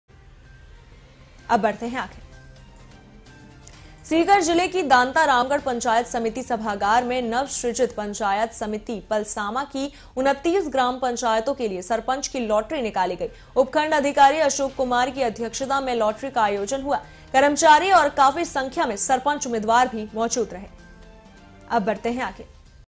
Sikar : दांतारामगढ़ पंचायत समिति सभागार में निकाली गई 29 ग्राम पंचायतों की लॉटरी
1. Jaipur : राजस्थान में नहीं लागू होगा CAA_NRC, सीएम अशोक गहलोत ने किया साफ
2. Sikar : दांतारामगढ़ पंचायत समिति सभागार में निकाली गई 29 ग्राम पंचायतों की लॉटरी
3. Barmer : भेड़ बकरी चराते-चराते शख्स बन गया गोल्डन मैन, शरीर पर है एक किलो सोना
4. Bikaner : पुलिस मोटर ड्राइविंग स्कूल में हुआ दीक्षांत समारोह, 391 कांस्टेबल बेड़े में शामिल
5. Baran : NH-27 पर गाय को बचाने के चक्कर में नहर में गिरा ट्रोला, बाल-बाल बचे लोग
6. Jodhpur में विरोध प्रदर्शन के बाद पथराव, पुलिस ने भांजी लाठियां, अतिरिक्त पुलिस बल तैनात
7. Jhunjhunu : ATM कार्ड बदलने का आरोपी भीड़ के हत्थे चढ़ा, उसे पिटती देखती रही पुलिस
8. Churu : ICICI बैंक में दिनदहाड़े 33.77 लाख की डकैती, पुलिस ने 6 बदमाशों को दबोचा, 4 हथियार बरामद
9. Udaipur : गाय ने बाइक को मारी टक्कर, नीचे गिरी दो महिलाओं और युवक को ट्रैक्टर ने कुचला, दो की मौत